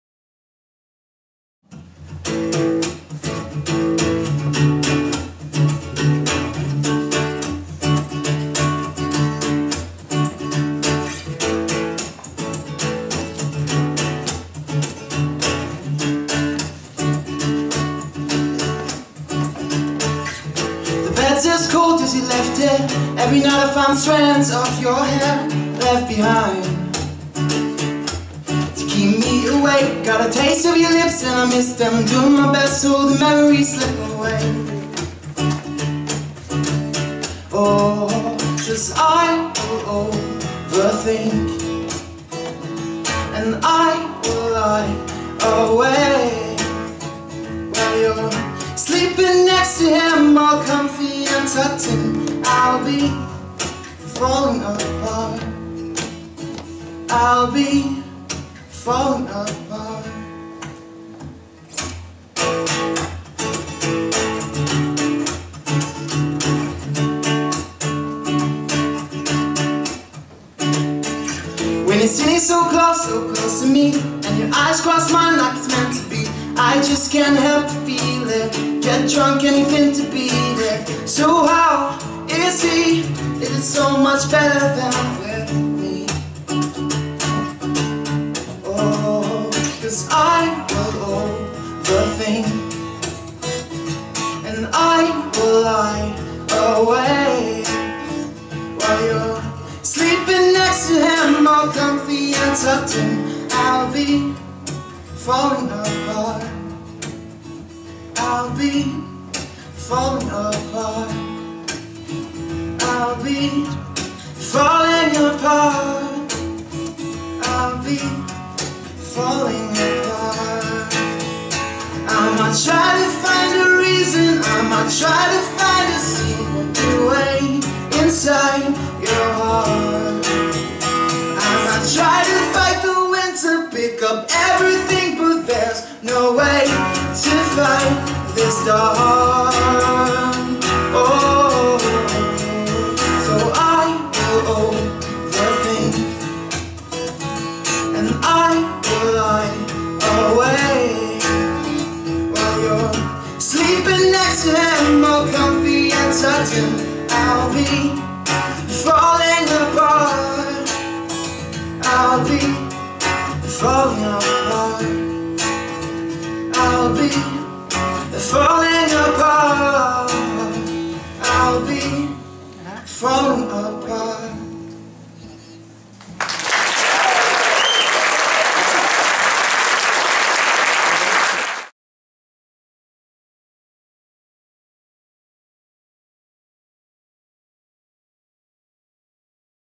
Philippe Clément - Concert des élèves décembre 2012
École de musique Sonart de Gatineau
Élève de guitare de Justin St-Onge